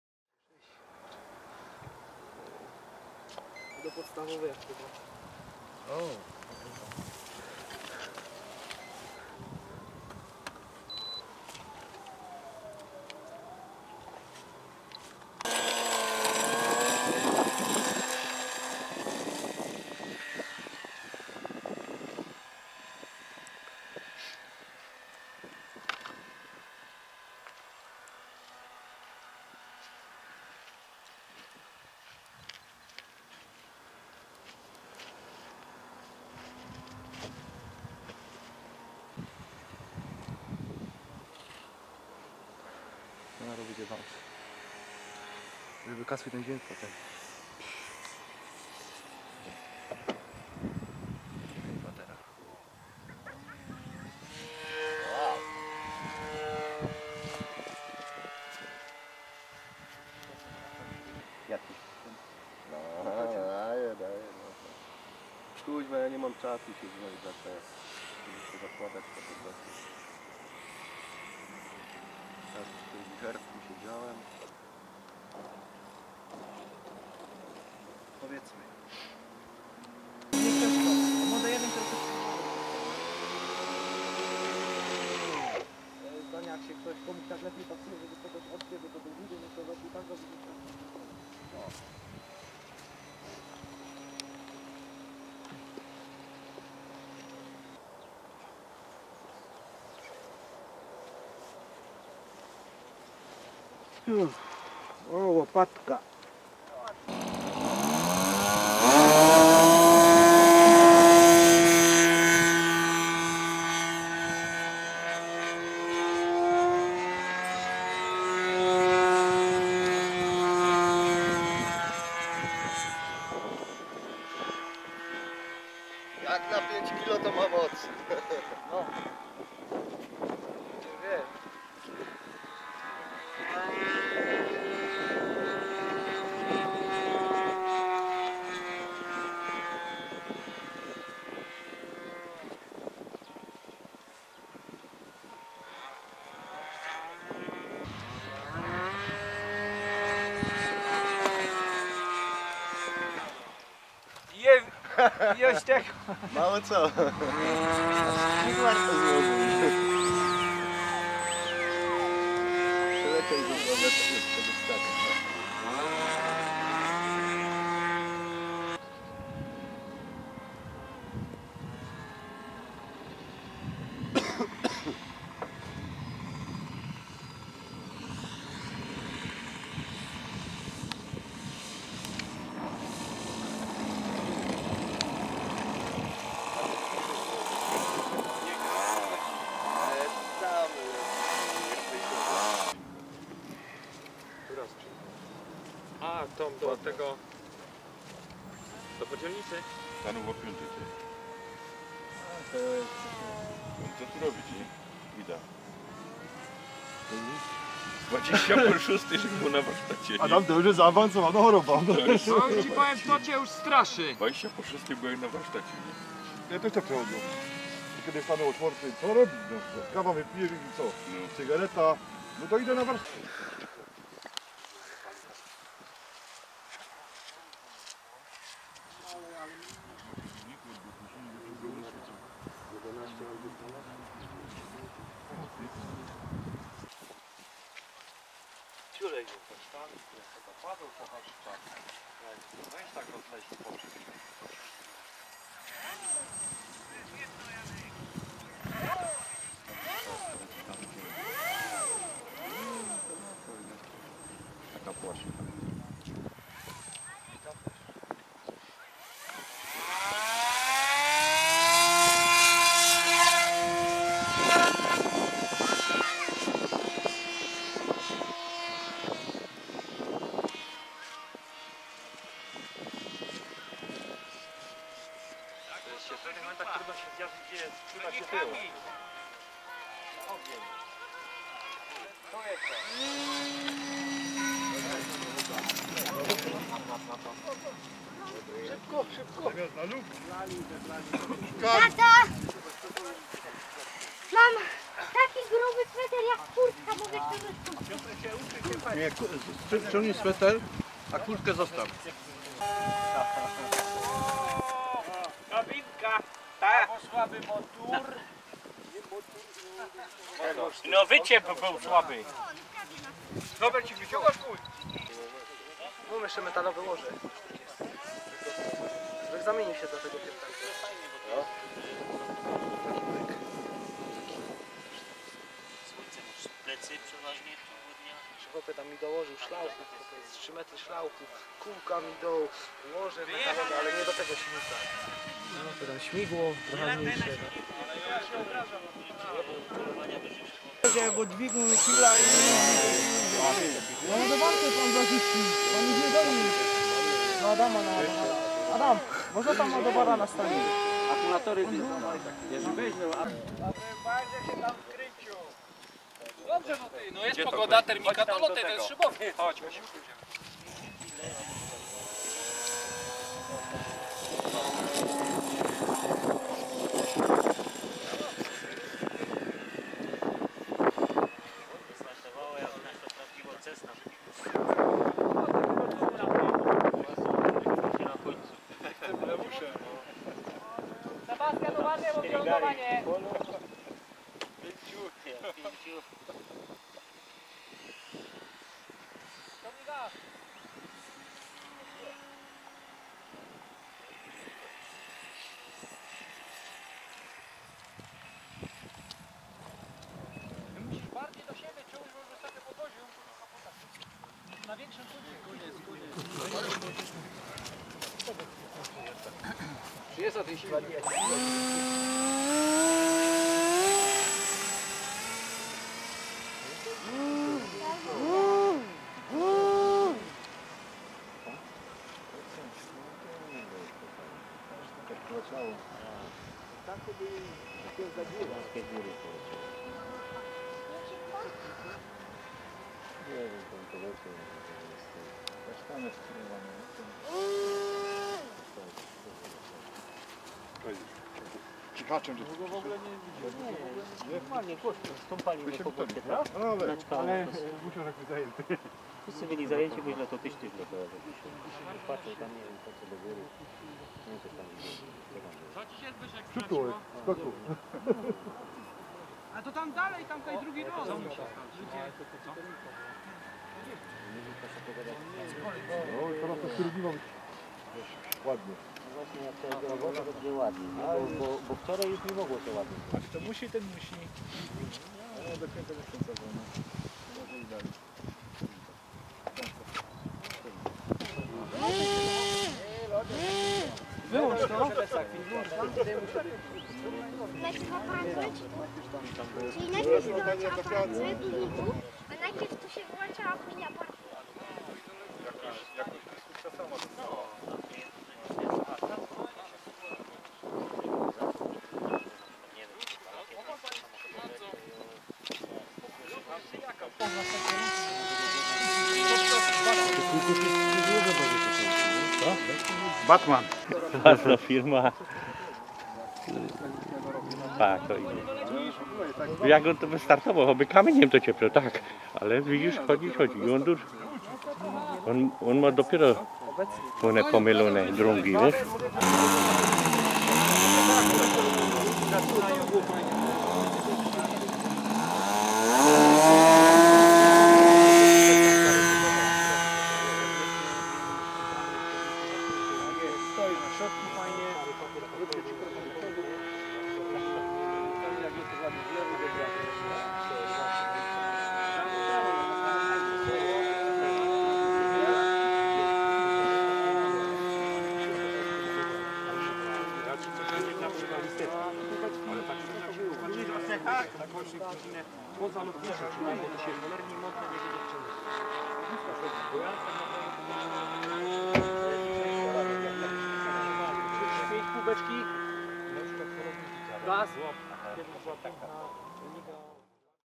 Lotnisko klubu "Gwarek" - 15 marzec 2015r.
Relacja z dzisiejszych lotów ;-)